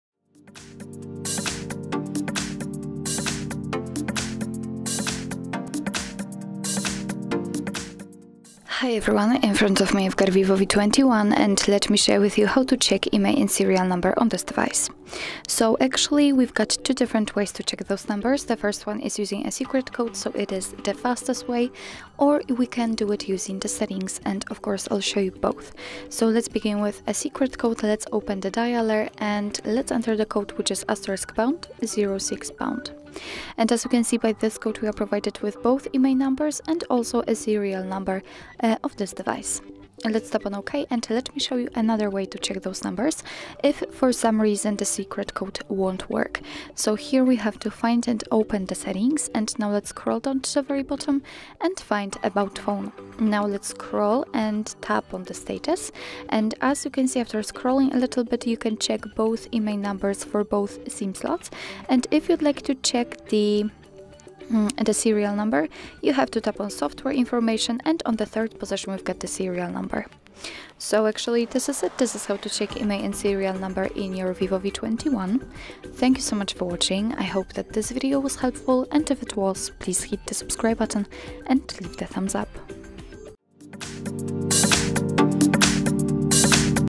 How to Check IMEI & Serial Number on VIVO V21 – Find Unique Numbers
Learn more about VIVO V21
Check this tutorial if you can't locate your IMEI and Serial Number on your VIVO V21. Our specialist will show you how to enter the dial pad code so that these numbers pop up on your phone. You can also find out how to enter the setting and how to find the full specs. Visit our IMEI.Info page if you wish to check the IMEI of your device.